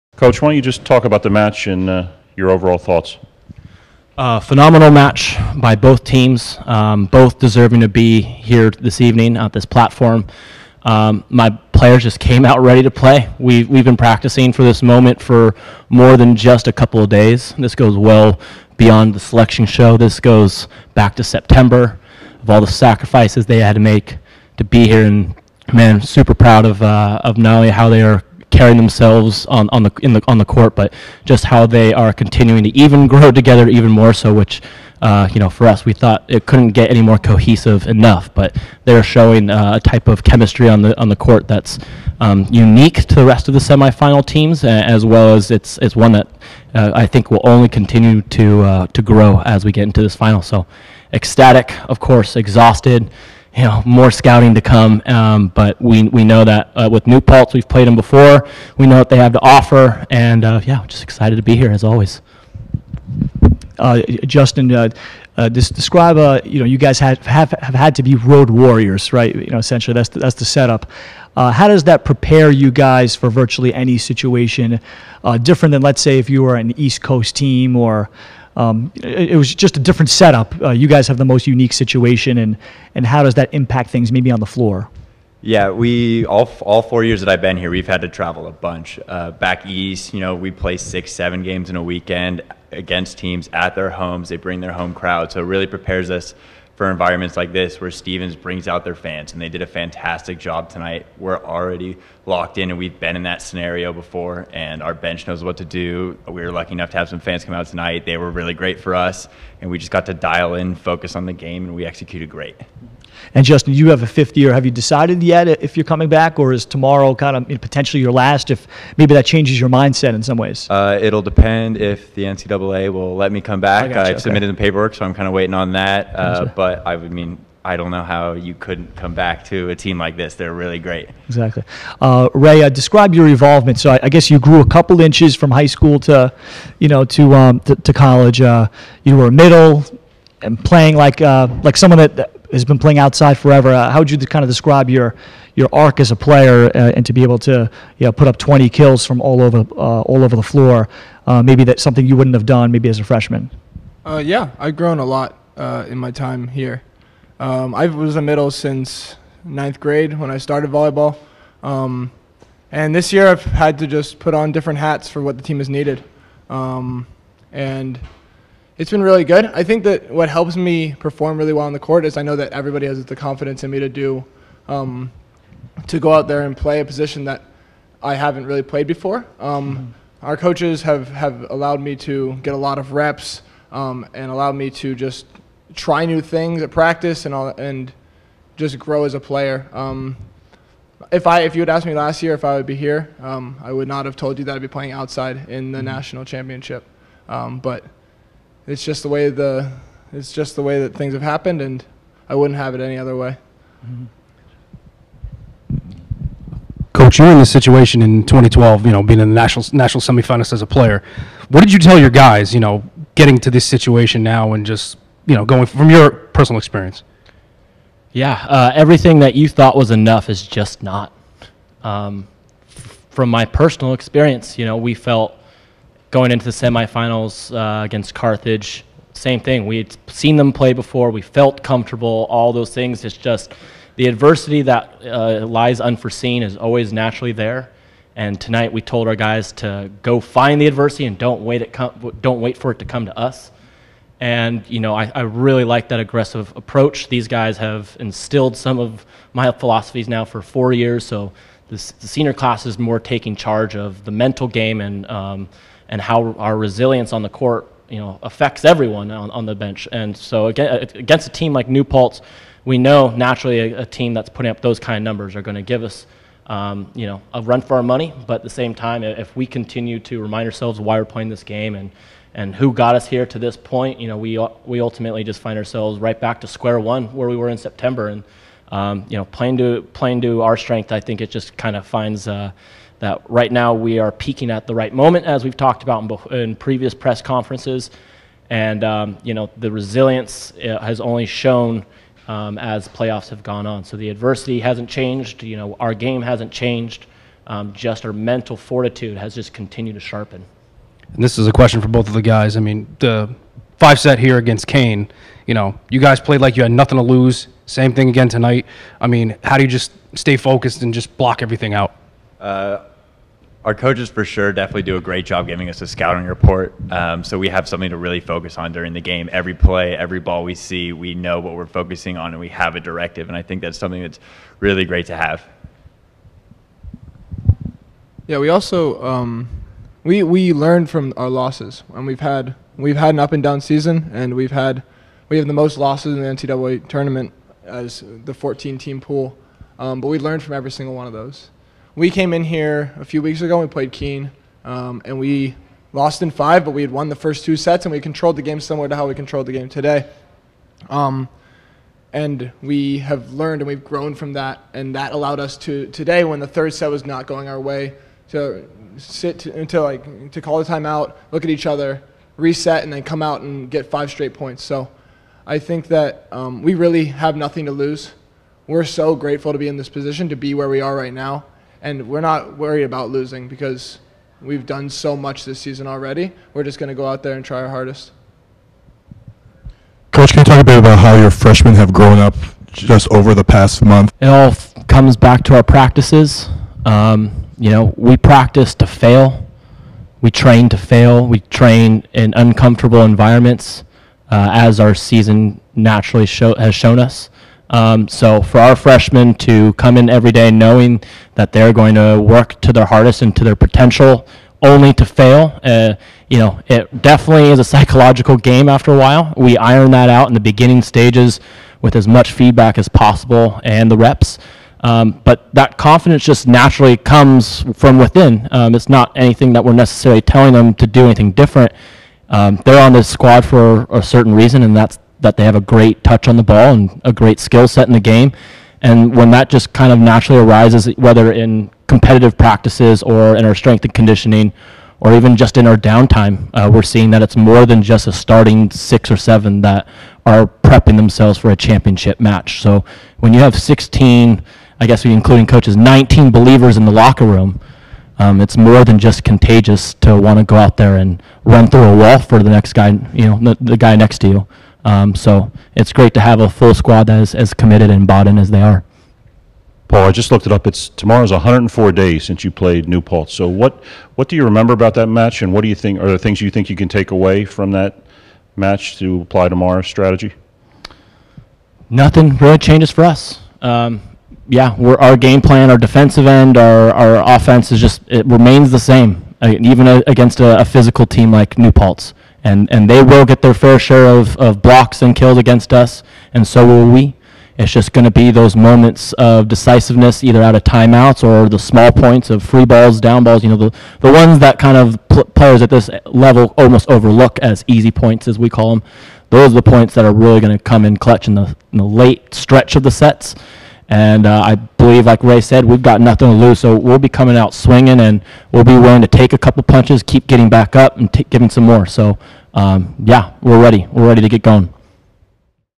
NCAA Div III Men's Volleyball Semifinal - UC Santa Cruz
Listen to Head Coach Paul Leon and student-athletes Justin Fortner and Raymond Cascio discuss their semifinal win over Stevens.  NCAA Div III Men's Volleyball Semifinal - UC Santa Cruz